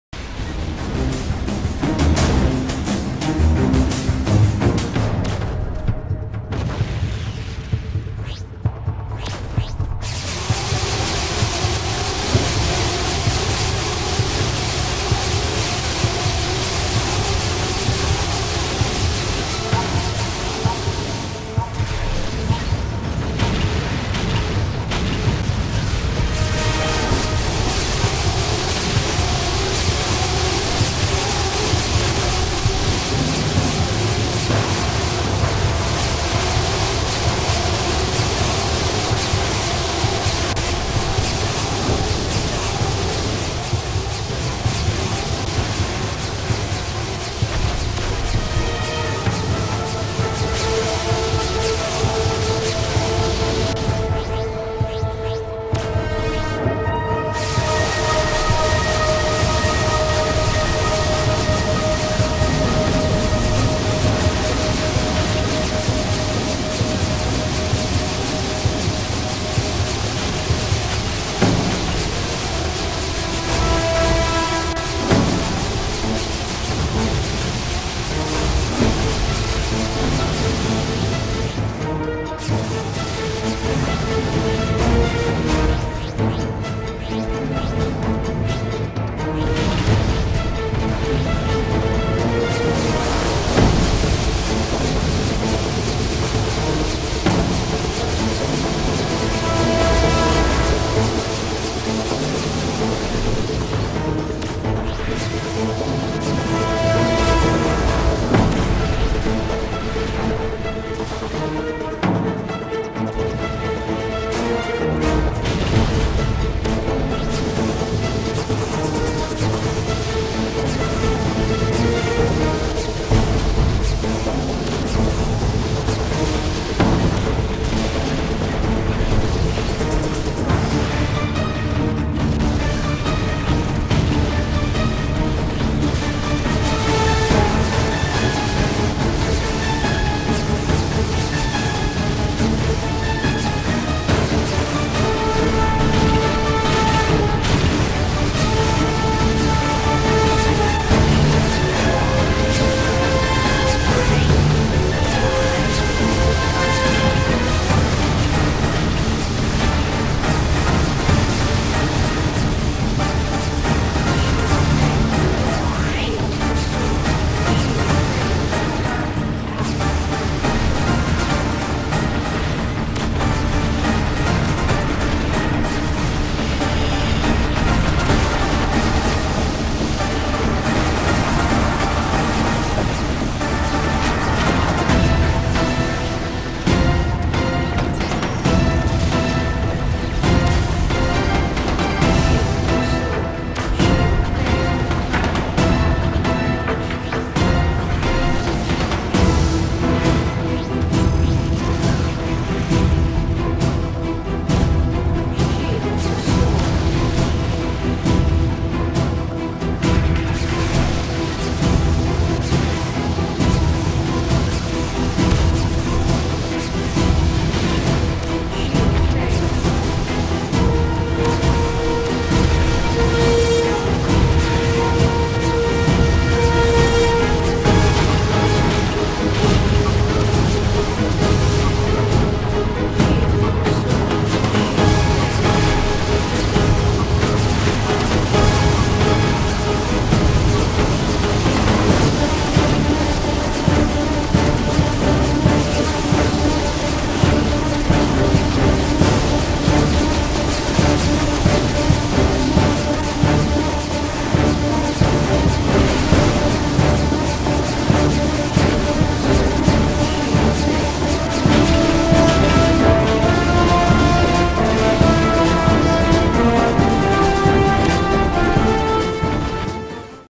Nomads invading Minor
The first major Nomad fleet attacking Omicron Minor during the BHG, Nomad, Order event.